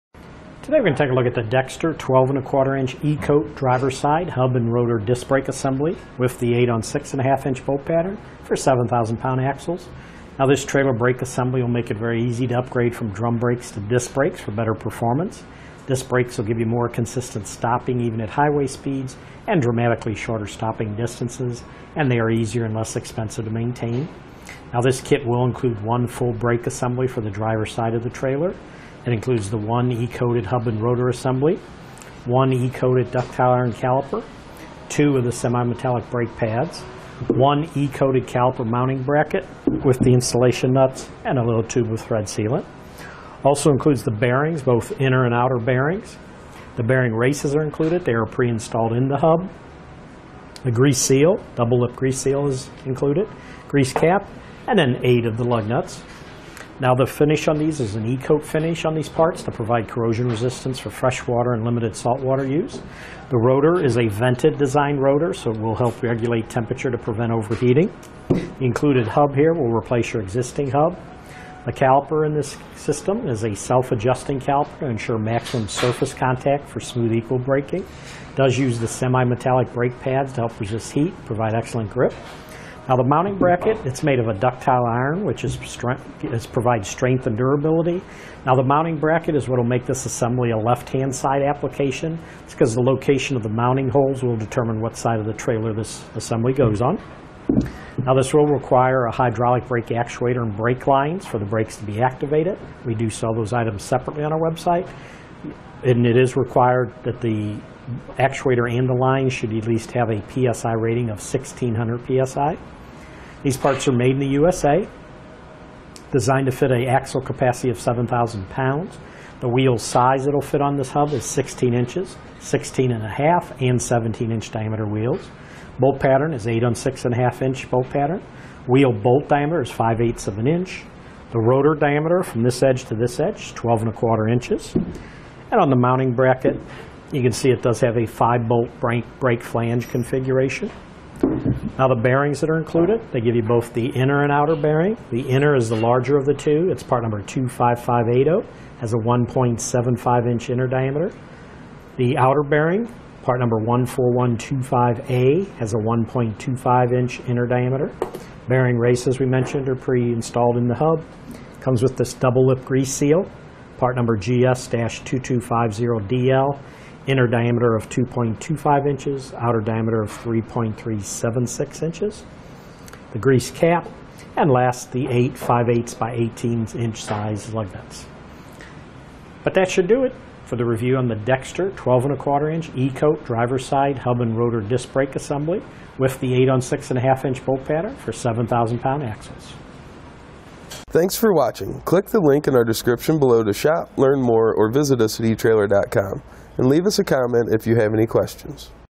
etrailer | Dexter Axle Trailer Brakes K71-694-416-14 Review
Speaker 1:          Today we're going to take a look at the Dexter 12 1/4 inch E-Coat Driver's Side Hub and Rotor Disc Brake Assembly with the 8 on 6 1/2 inch bolt pattern for 7,000 pound axles. Now this trailer brake assembly will make it very easy to upgrade from drum brakes to disc brakes for better performance. Disc brakes will give you more consistent stopping even at highway speeds and dramatically shorter stopping distances, and they are easier and less expensive to maintain.Now this kit will include one full brake assembly for the driver's side of the trailer. It includes the one E-Coated hub and rotor assembly. One E-Coated duct tower and caliper, two of the semi-metallic brake pads, one E-Coated caliper mounting bracket with the installation nuts and a little tube of thread sealant.Also includes the bearings, both inner and outer bearings. The bearing races are included.

They are pre-installed in the hub. The grease seal, double up grease seal is included, grease cap, and then eight of the lug nuts.Now, the finish on these is an E-Coat finish on these parts that provide corrosion resistance for fresh water and limited salt water use. The rotor is a vented design rotor. So it will help regulate temperature to prevent overheating. The included hub here will replace your existing hub.

It does use the semi-metallic brake pads to help resist heat and provide excellent grip.Now, the mounting bracket. It's made of a dove tail iron, which provides strength and durability. Now the mounting bracket is what will make this assembly a left-hand side application. It's because of the location of the mounting holes that will determine what side of the trailer this assembly goes on.

Now this will require a hydraulic brake actuator and brakes lines for the brakes to be activated. We do sell those items separately on our website. And it is required that the actuator and the lines should at least have a PSI rating of 1,600 PSI.These parts are made in the U.S.A. Designed to fit an axle capacity of 7,000 pounds. The wheel size that will fit on this hub is 16 inches, 16 1/2, and 17 inch diameter wheels.

Bolt pattern is 8 on 6 1/2 inch bolt pattern. Wheel bolt diameter is 5/8 of an inch. The rotor diameter from this edge to this edge, 12 1/4 inches. And on the mounting bracket, you can see it does have a five bolt brake flange configuration.Now the bearings that are included, they give you both the inner and outer bearing. The inner is the larger of the two. It's part number 25580. It has a 1.75 inch inner diameter. The outer bearing, part number 14125A has a 1.25 inch inner diameter. Bearing races, as we mentioned, are pre-installed in the hub. It comes with this double lipped grease seal, part number GS-2250DL, inner diameter of 2.25 inches. Outer diameter of 3.376 inches. The grease cap and last, the 8 5/8 by 18 inch size lug nuts.But that should do it for the review on the Dexter 12 1/4 inch E-Coat Driver's Side Hub and Rotor Disc Brake Assembly with the 8 on 6 1/2 inch bolt pattern for 7,000 pound axles.